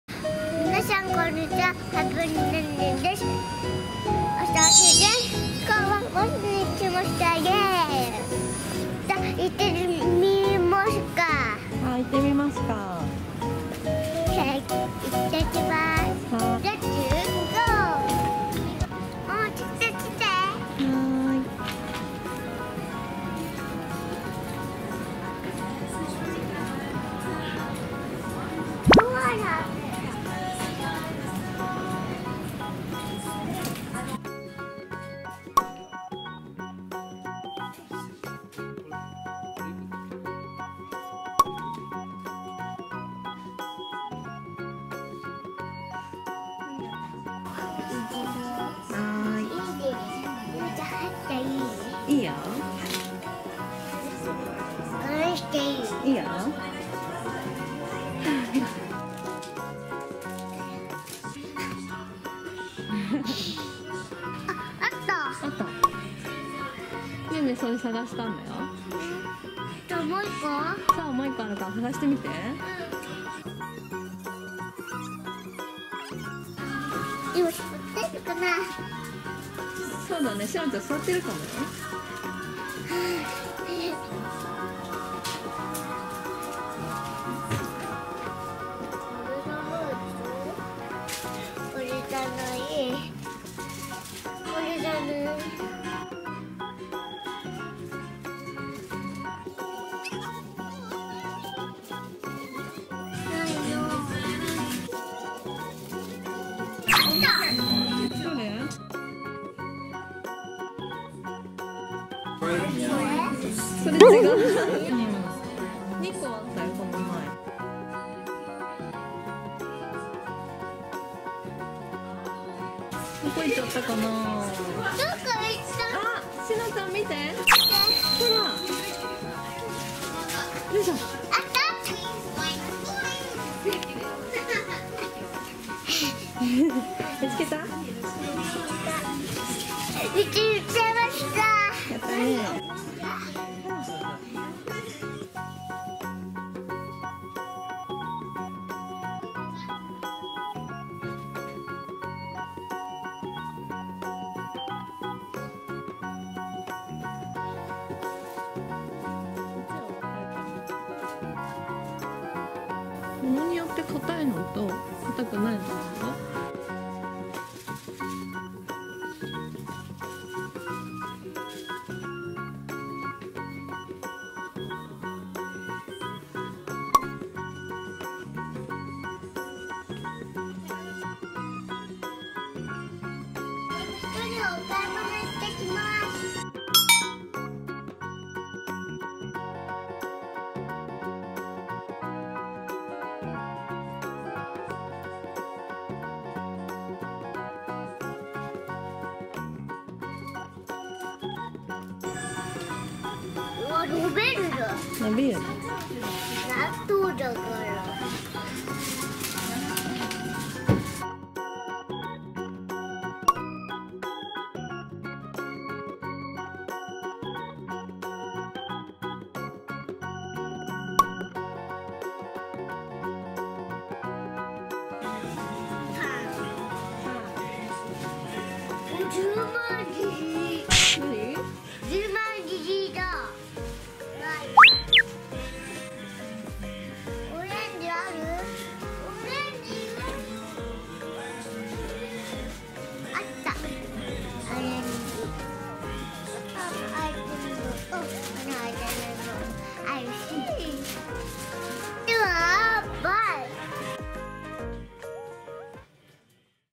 【原宿】モッシュ♪しのちゃん初の新店舗でハイテンション！100万円プールは貸切状態♪
クリスマス前の原宿モッシュに行ってきた時に撮影したものです。
小学校より１日先に幼稚園が冬休みに入ったので、いちごのプール入りたい！！って言ってたしのちゃんをモッシュに連れていく事が出来ました♪さゆちゃんがいちごのプールに入っていたのを動画で見ていたので、ほんとずっと行きたくてしょうがなかったみたいですw

ママも買いたいスクイーズがあったので、平日は空いてそうだと思い行ってきたら、プールは貸切状態でした！
ずっと浸かりっぱなしでしたよw
匂いも良いし、触り心地も最高な場所です(*^^*)

購入したスクイーズはまた紹介しますね♪

UUUM株式会社  ハピバ二channel宛